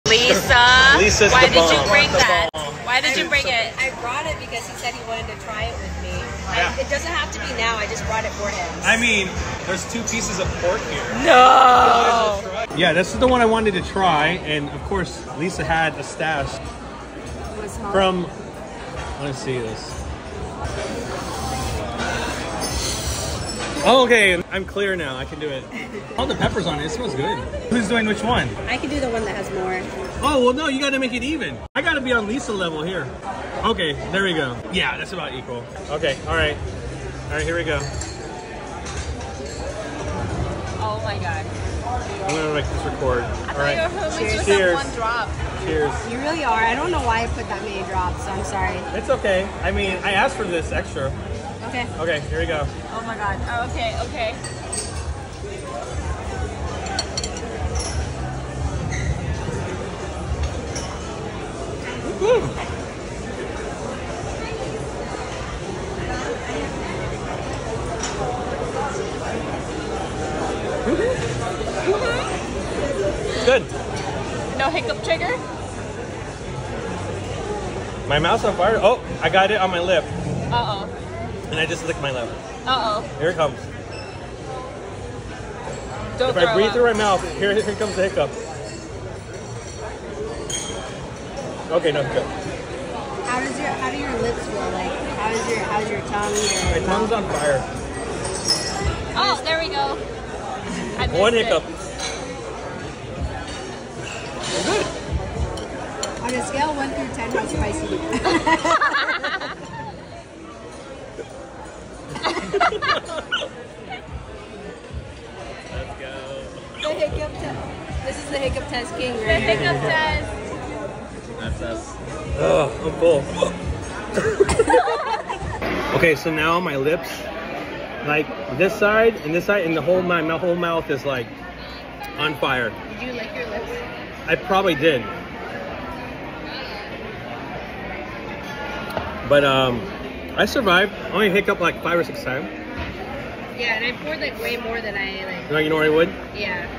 Full Reaction! Da' Bomb Hot Sauce with @LisaNguyen 🔥
This is my full reaction to trying Da' Bomb Hot Sauce with @LisaNguyen while @Jeanelleats films and commentates. We talk about Doug having hiccups to hot sauce and I can get them as well to spicy food. Watch my spicy review at the end 🔥 Thanks to Lisa for gifting me Da' Bomb Hot Sauce!

🌶 DaBomb Hot Sauce As seen on Hot Ones dabombhotsauce 🔥

Filmed by @Jeanelleats iPhone